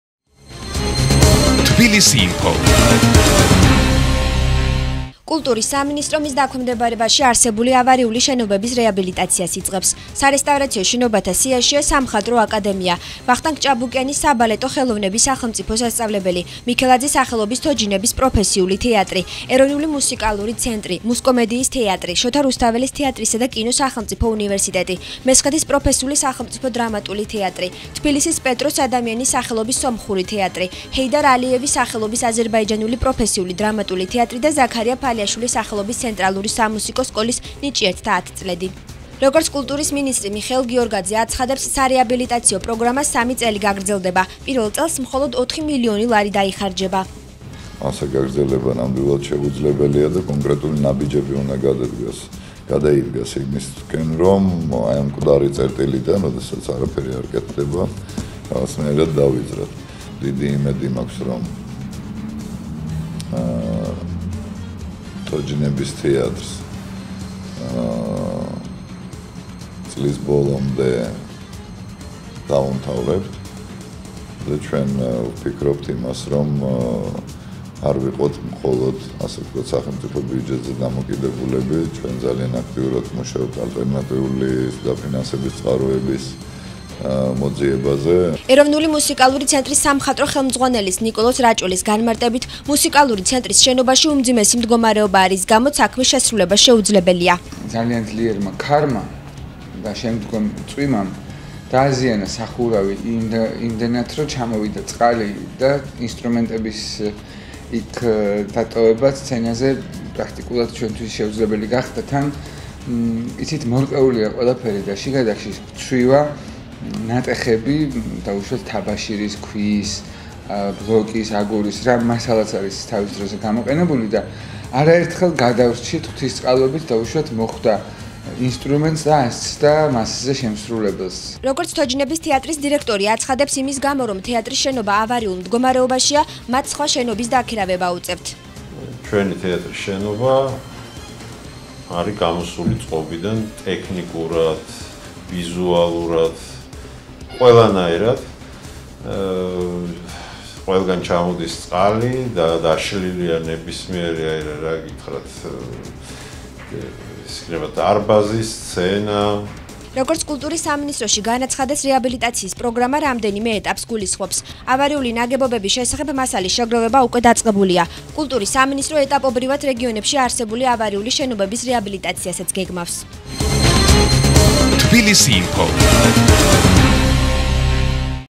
კულტურის სამინისტრო ავარიული შენობების რეაბილიტაციას იწყებს
თოჯინების თეატრი და ეროვნული მუსიკალური ცენტრი სხვა კულტურულ დაწესებულებებთან ერთად მრავალი წლის შემდეგ რეაბილიტირდება…